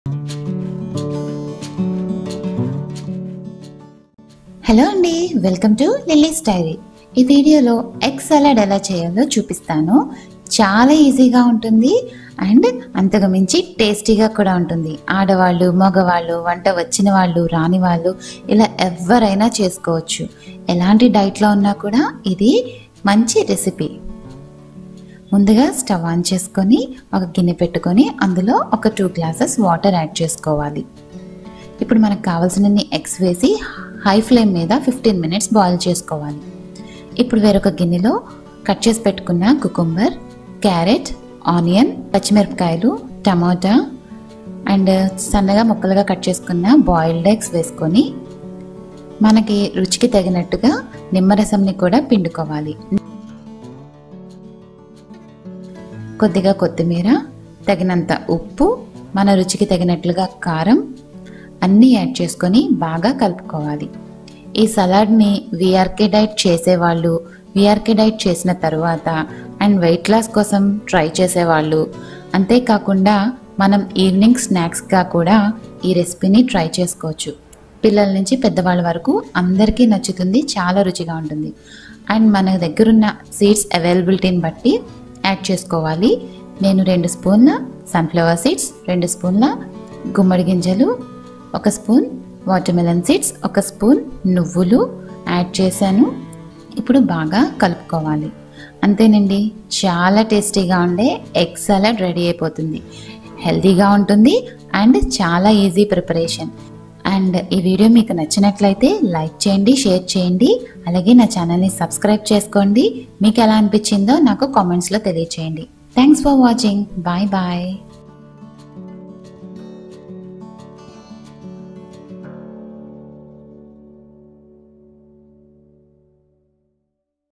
WEIGHT LOSS RECIPE || EGG SALAD || LILLY'S DIARY
Hello everyone in this video i'm sharing a WEIGHT LOSS RECIPE , EGG SALAD . Everyone  can try this recipe  specially for weight loss , weight maintaining , vrk diet followers , post vrk diet anyone can try this tasty recipe.
Try this recipe and leave your valuable comments below.